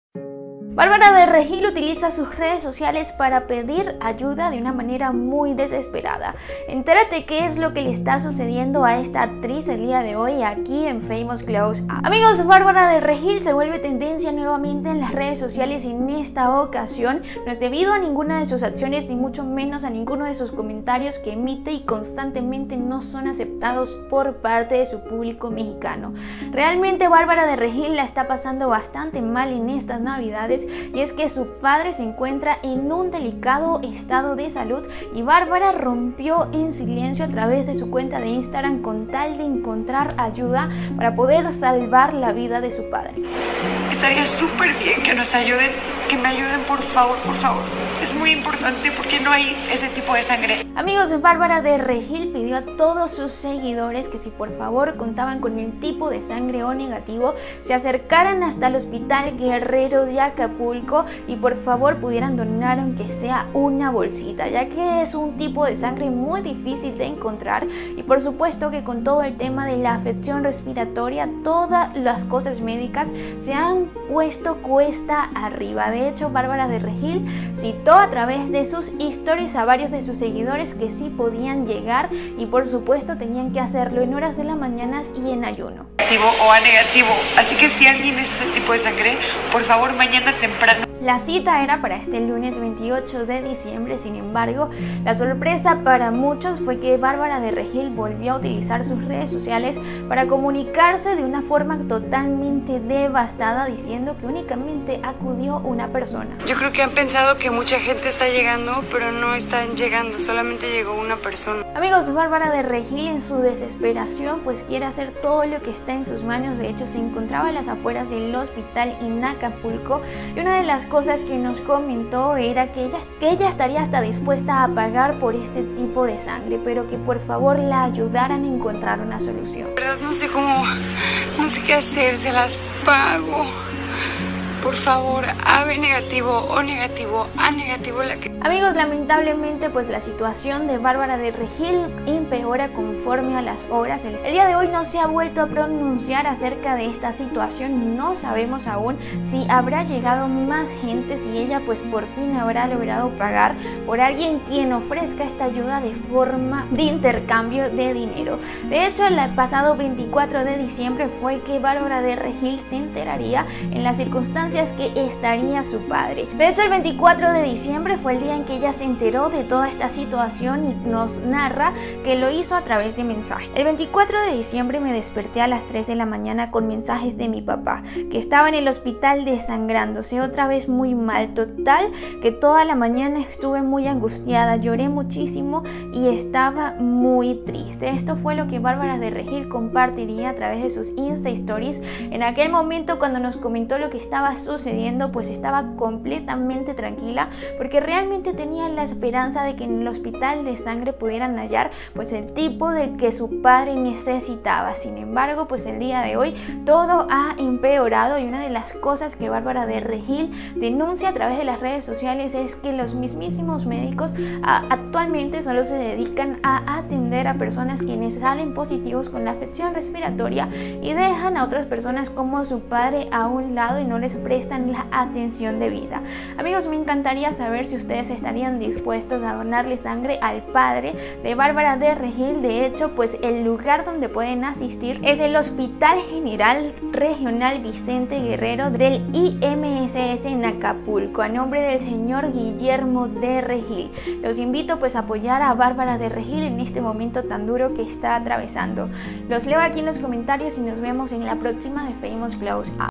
🔴¡ 𝗛𝗮𝗰𝗲 𝗨𝗻𝗮𝘀 𝗛𝗼𝗿𝗮𝘀 ! Barbara de Regil 𝗣𝗜𝗗𝗘 𝗢𝗥𝗔𝗖𝗜𝗢𝗡𝗘𝗦 Noticias de Ultima Hora !
La actriz Bárbara de Regil tuvo una Navidad bastante acontecida. A través de su cuenta en Instagram reveló que su familia estaba pasando por un momento bastante difícil.